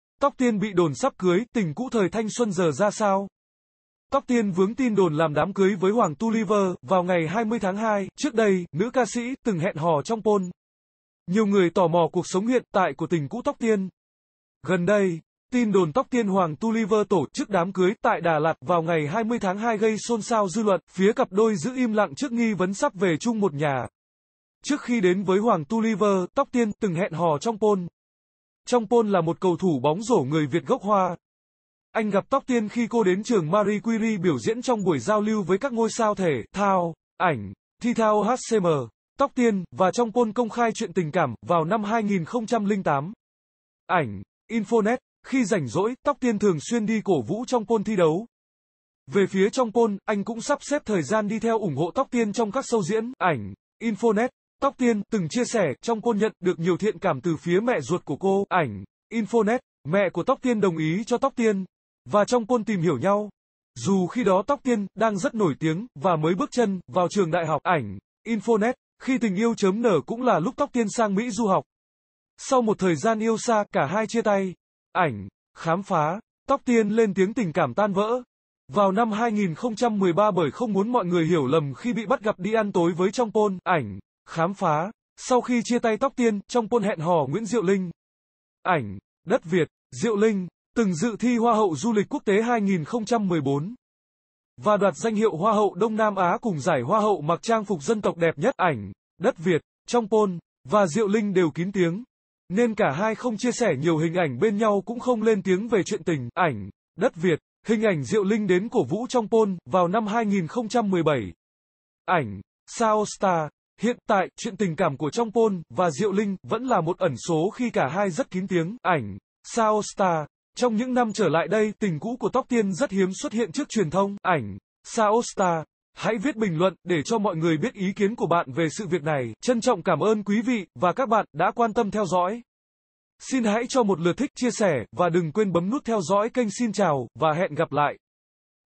Tóc Tiên bị đồn sắp cưới, tình cũ thời thanh xuân giờ ra sao?
Tóc Tiên vướng tin đồn làm đám cưới với Hoàng Touliver vào ngày 20/2. Trước đây, nữ ca sĩ từng hẹn hò Chong Paul. Nhiều người tò mò cuộc sống hiện tại của tình cũ Tóc Tiên.Gần đây, tin đồn Tóc Tiên - Hoàng Touliver tổ chức đám cưới tại Đà Lạt vào ngày 20/2 gây xôn xao dư luận
Phía cặp đôi giữ im lặng trước nghi vấn sắp về chung một nhà
Trước khi đến với Hoàng Touliver, Tóc Tiên từng hẹn hò Chong Paul.Chong Paul là một cầu thủ bóng rổ người Việt gốc Hoa
Anh gặp Tóc Tiên khi cô đến trường Marie Curie biểu diễn trong buổi giao lưu với các ngôi sao thể thao
Ảnh: Thethaohcm
Tóc Tiên và Chong Paul công khai chuyện tình cảm vào năm 2008
Ảnh: Infonet
Khi rảnh rỗi, Tóc Tiên thường xuyên đi cổ vũ Chong Paul thi đấu
Về phía Chong Paul, anh cũng sắp xếp thời gian đi theo ủng hộ Tóc Tiên trong các show diễn
Ảnh: Infonet
Tóc Tiên từng chia sẻ, Chong Paul nhận được nhiều thiện cảm từ phía mẹ ruột của cô
Ảnh: Infonet
Mẹ của Tóc Tiên đồng ý cho Tóc Tiên và Chong Paul tìm hiểu nhau dù khi đó Tóc Tiên đang rất nổi tiếng và mới bước chân vào trường đại học
Ảnh: Infonet
Khi tình yêu chớm nở cũng là lúc Tóc Tiên sang Mỹ du học
Sau một thời gian yêu xa, cả hai chia tay
Ảnh: Khám phá
Tóc Tiên lên tiếng tình cảm tan vỡ vào năm 2013 bởi không muốn mọi người hiểu lầm khi bị bắt gặp đi ăn tối với Chong Paul
Ảnh: Khám phá
Sau khi chia tay Tóc Tiên, Chong Paul hẹn hò Nguyễn Diệu Linh
Ảnh: Đất Việt
Diệu Linh từng dự thi Hoa hậu Du lịch Quốc tế 2014 và đoạt danh hiệu Hoa hậu Đông Nam Á cùng giải Hoa hậu mặc trang phục dân tộc đẹp nhất
Ảnh: Đất Việt
Chong Paul và Diệu Linh đều kín tiếng nên cả hai không chia sẻ nhiều hình ảnh bên nhau cũng không lên tiếng về chuyện tình
Ảnh: Đất Việt
Hình ảnh Diệu Linh đến cổ vũ Chong Paul vào năm 2017
Ảnh: Saostar
Hiện tại, chuyện tình cảm của Chong Paul và Diệu Linh vẫn là một ẩn số khi cả hai rất kín tiếng
Ảnh: Saostar
Trong những năm trở lại đây, tình cũ của Tóc Tiên rất hiếm xuất hiện trước truyền thông
Ảnh: Saostar.